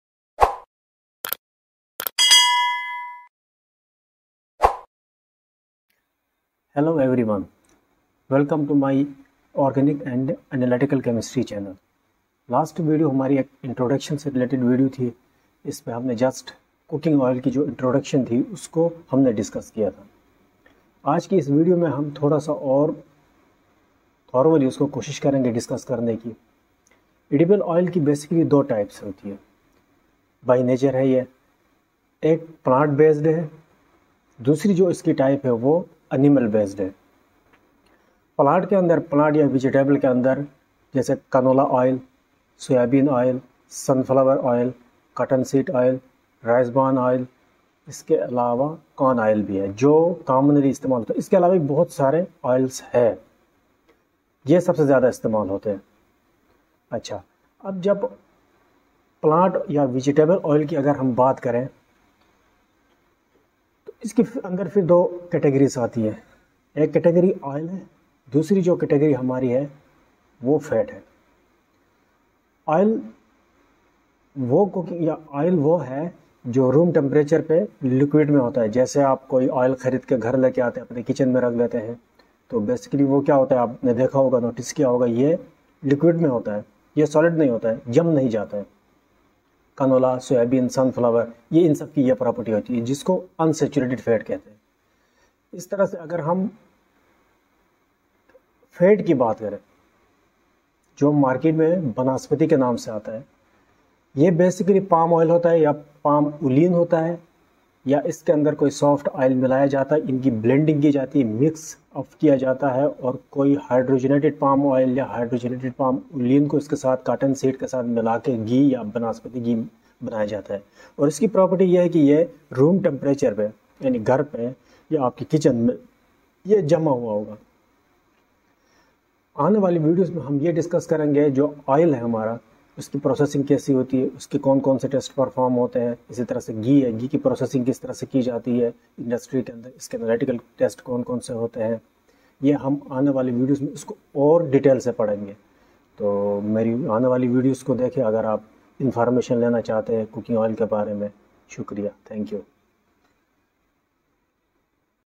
Which Cooking Oil is Best For Health (organic Chemistry)
Edible oils consist of about 96% triacylglycerides, composed of different fatty acids. Some other compounds or groups of compounds, such as free fatty acids, phospholipids, phytosterols, tocopherols, other antioxidants or waxes, can also be found. Fatty acids, free or bound to glycerol are susceptible to oxidative processes resulting in a wide range of volatile and non-volatile degradation products. Therefore one of the major challenges for the oil processing industry is to maintain the high quality of the product after processing until use by the consumer. However, the oxidative stability of edible oils not only depends on conditions during storage, but also the history of the raw material and the processing steps involved.